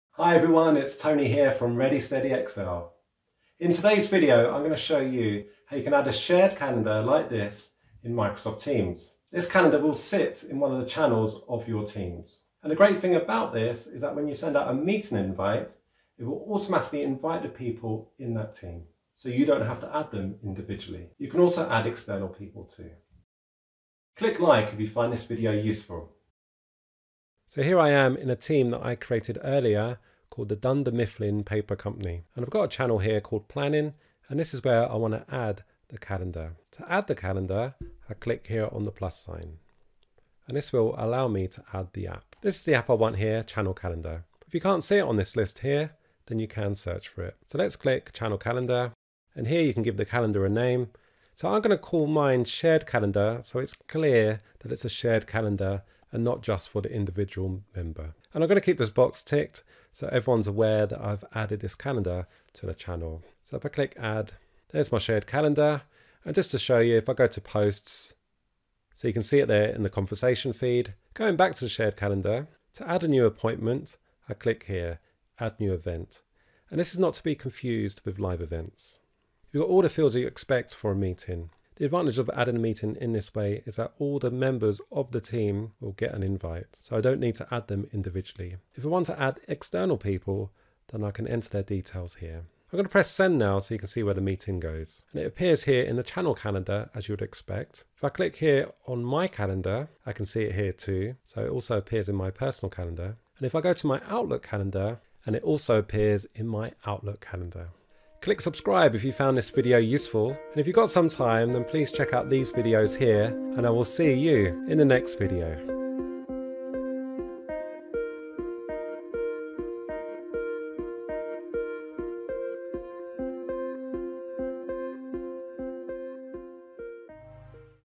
How to add a shared channel calendar in Microsoft Teams
Learn how to add a shared calendar to Microsoft Teams. By adding a Channel Calendar to your Team, members will be able to add meetings and appointments to the Channels for all members to see. In this video, I will show you how to set up a shared calendar in MS Teams.

☕ Help top up my caffeine levels and get a mention in my next video:

🛠 Tools
Strategies in this book helped grow my Channel